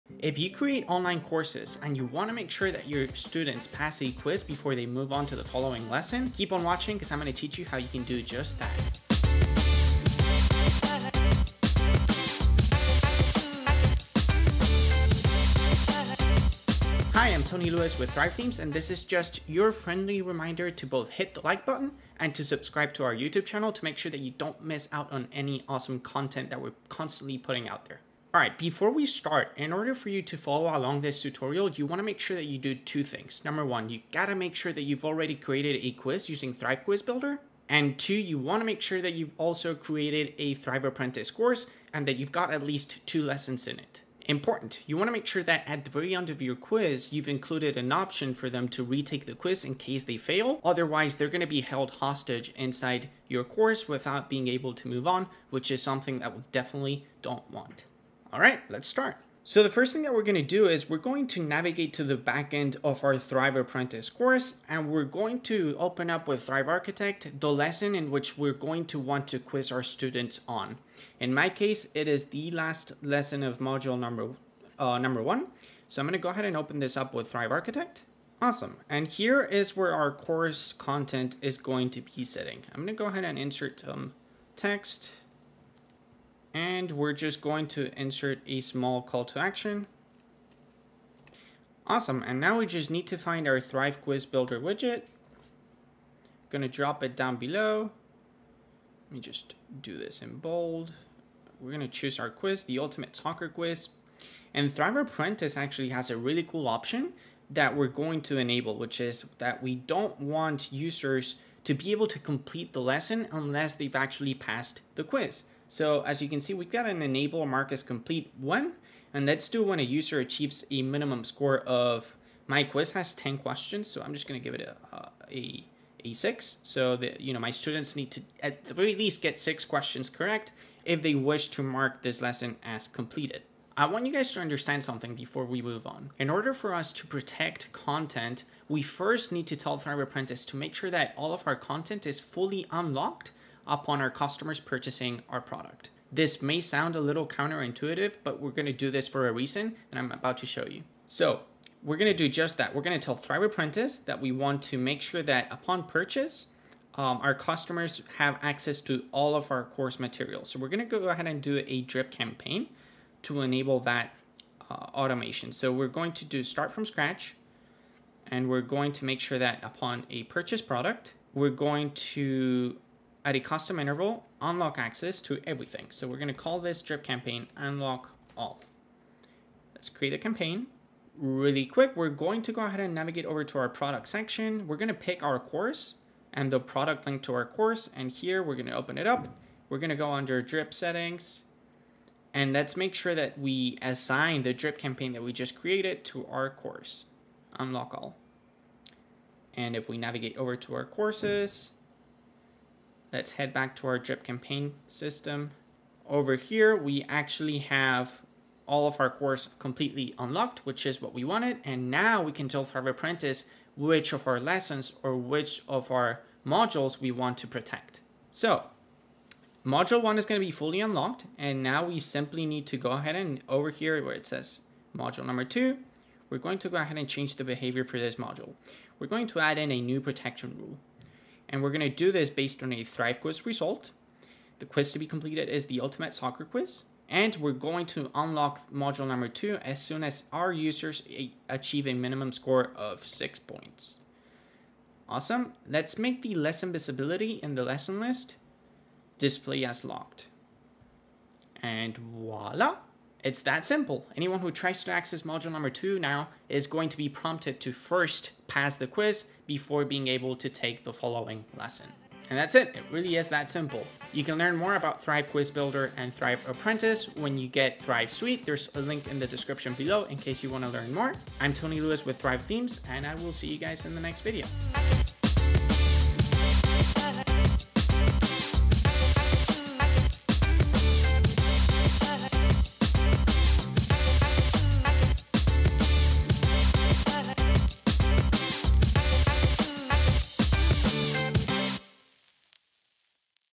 How to Test Your Students Before They Can Move on to the Next Lesson in Your Online Course
Wouldn’t it be nice if your students had to PASS a quiz before they could move on to the next lesson inside your online course?

In this video, you’ll learn how to use Thrive Quiz Builder quizzes to restrict access to more advanced lessons in your Thrive Apprentice built online courses.

Time Stamps:

00:00 - Intro
00:16 - What to Prepare
00:44 - Important Online Quiz Settings
00:59 - Add a Quiz to a Thrive Apprentice Course Lesson
02:30 - Create Your Drip Campaign
04:22 - Your Turn to A Create a Quiz for Your Online Course

Would you consider implementing such quizzes to unlock more advanced course content?